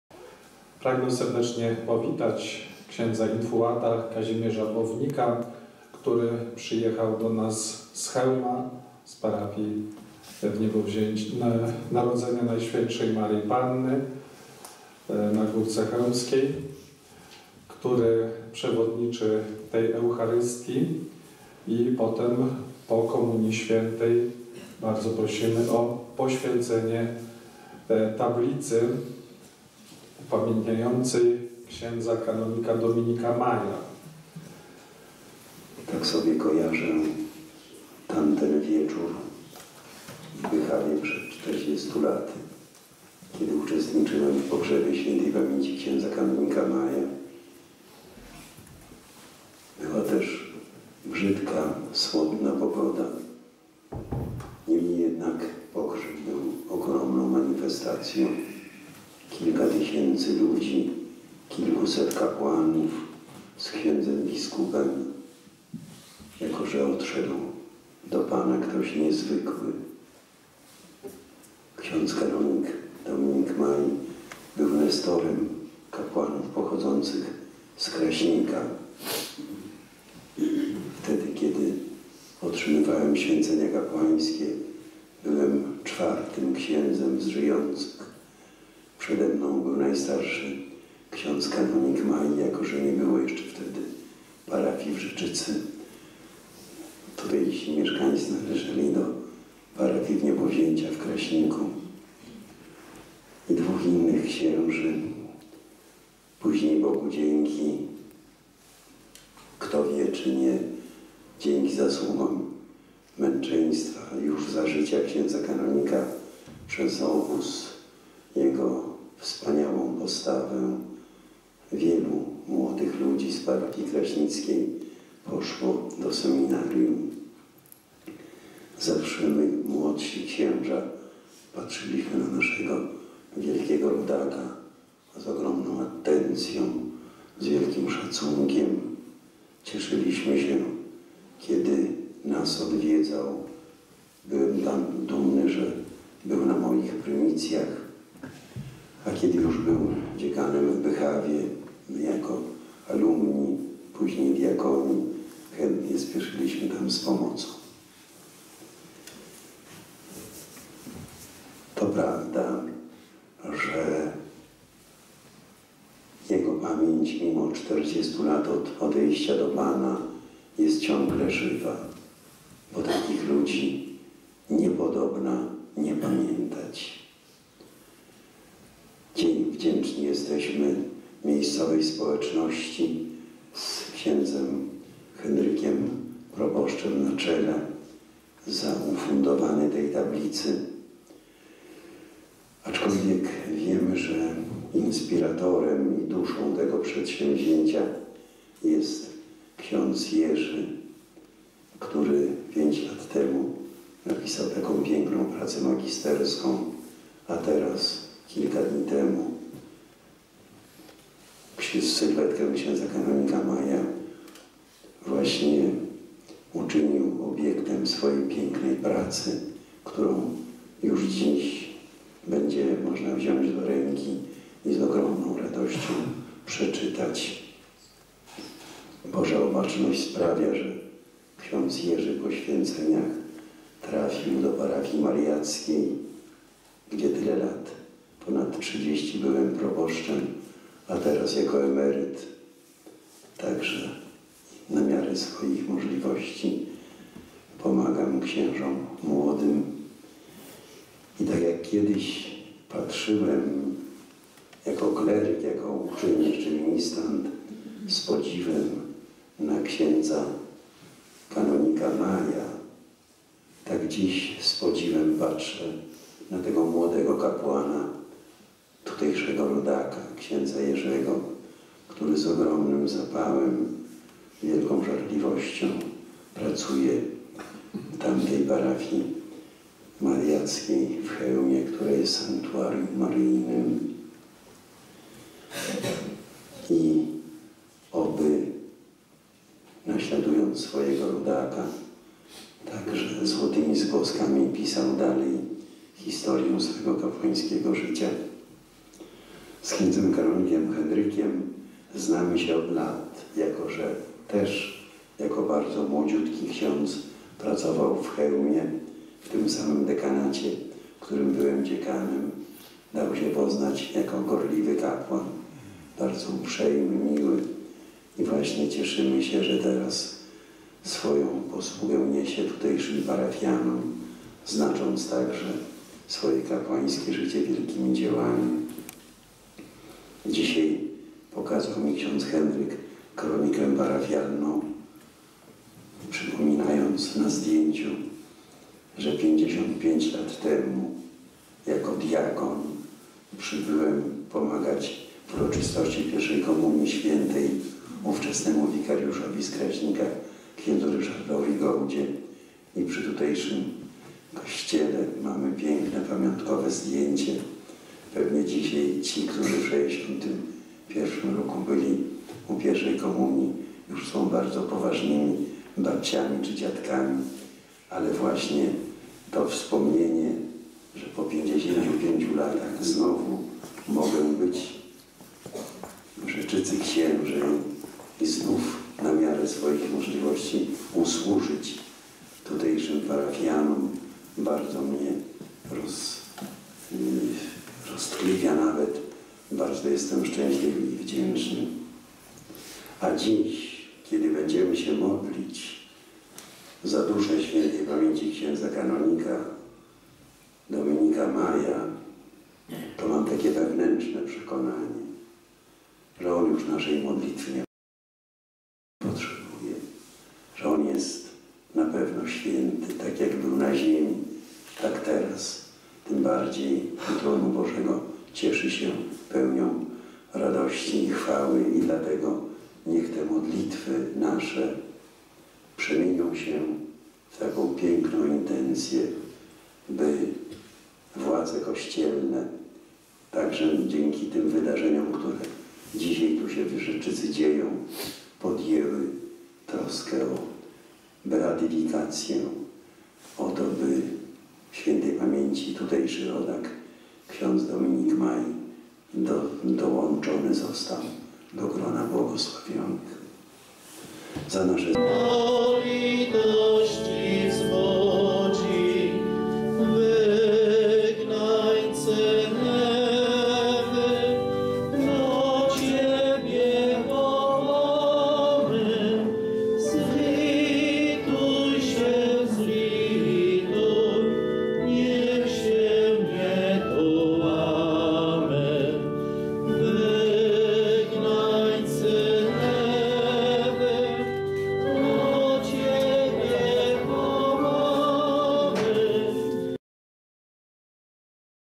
Poświęcenie tablicy pamiątkowej w 40. rocznicę śmierci ks Kan Dominika Maja
12 XI 2016r., w 40. rocznicę śmierci ks. Kan. Dominika Maja, w par. w Rzeczycy Księżej z której Ks Maj pochodził, Ks Infułat Kazimierz Bownik celebrował Mszę św w intencji jego rychłej beatyfikacji  i uroczyście poświęcił pamiątkową tablicę. Homilię wygłosił Ks Jerzy Krawczyk, autor książki "Ks. Dominik Maj - Kapłan Serca Bożego" a koncelebrował  Proboszcz Rzeczyckiej parafii Ks. Henryk Jargiło i Ks. Krzysztof Łaszcz SI